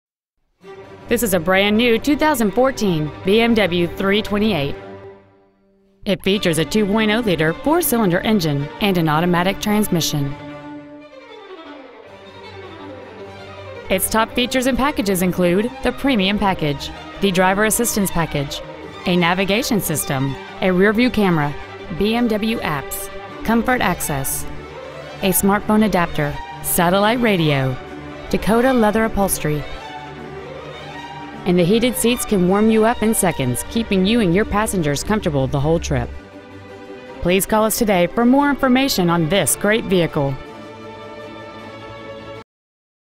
2014 BMW 328I Beverly Hills CA 90036
Year : 2014
 Make : BMW
 Model : 328I
 Engine : 2.0L I-4 cyl
 Trans . : Automatic
 Exterior : Mineral White Metallic

 Interior : Leather Dakota Saddle Brown exclusive stitching

 2014 BMW 328I Beverly Hills CA